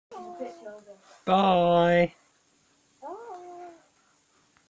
baby the parrot says bye bye
babys bye bye speak every time i leave the  house